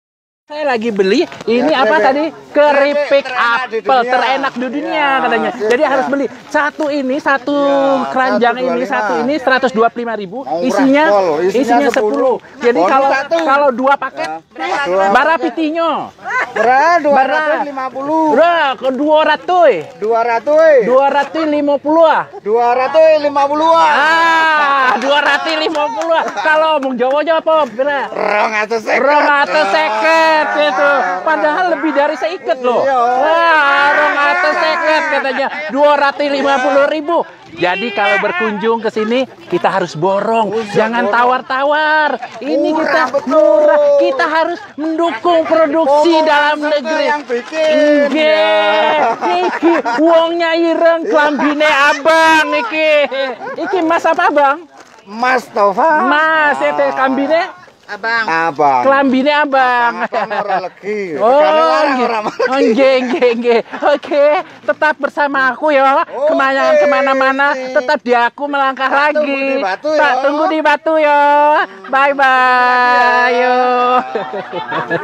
Serunya naik PAJERO (panas njobo jero)(6)
Pajero (panas njobo jero) adalah istilah yg dipergunakan untk angkutan desa ini, karna naik Pajero in panas luar dalam kita rasakan. Angkutan desa ini membawa kita ke Kebun Apel Mandiri di kota Batu Malang, merupakan wisata alam yang sangat mengesankan.